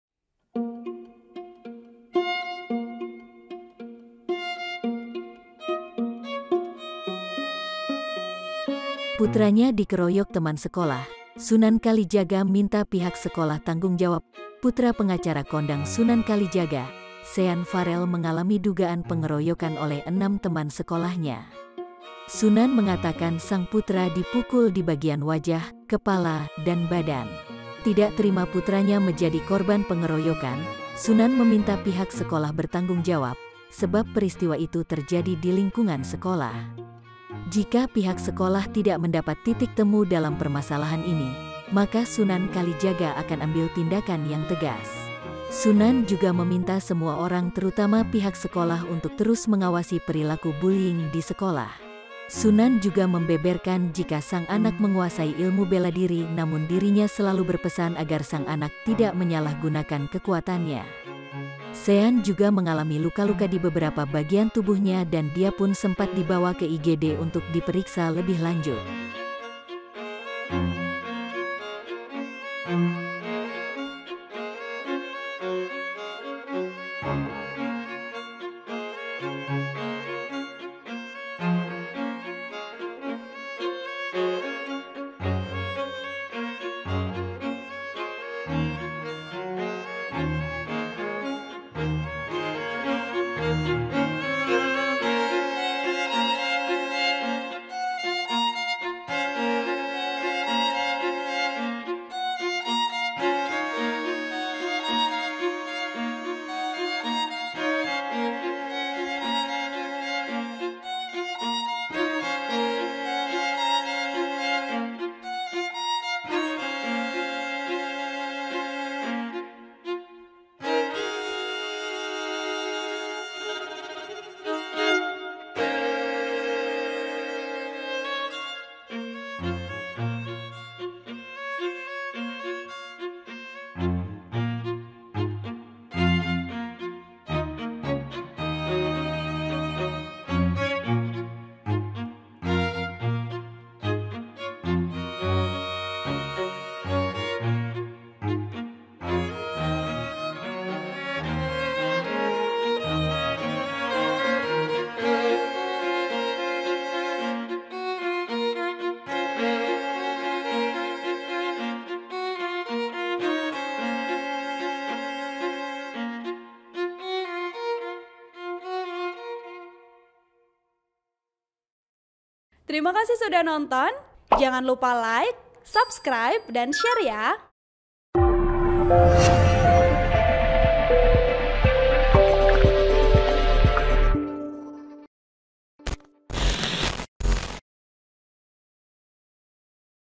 Putranya Dikeroyok Teman Sekolah, Sunan Kalijaga Minta Pihak Sekolah Tanggung Jawab
POS-KUPANG.COM - Putra pengacara kondang Sunan Kalijaga, Sean Farrel mengalami dugaan pengeroyokan oleh enam teman sekolahnya. Sunan mengatakan sang putra dipukul di bagian wajah, kepala, dan badan.

Tidak terima putranya mejadi korban pengeroyokan, Sunan meminta pihak sekolah bertanggung jawab, sebab peristiwa itu terjadi di lingkungan sekolah. Jika pihak sekolah tidak mendapat titik temu dalam permasalahan ini, maka Sunan Kalijaga akan ambil tindakan yang tegas.

Sunan juga meminta semua orang terutama pihak sekolah untuk terus mengawasi perilaku bullying di sekolah.

Sunan juga membeberkan jika sang anak menguasai ilmu bela diri namun dirinya selalu berpesan agar sang anak tidak menyalahgunakan kekuatannya. Sean juga mengalami luka-luka dibeberapa bagian tubuhnya dan dia pun sempat dibawa ke IGD untuk diperiksa lebih lanjut.
(POS-KUPANG.COM, Lenny)

Source     : lensa_berita_jakarta
VOP         : Lenny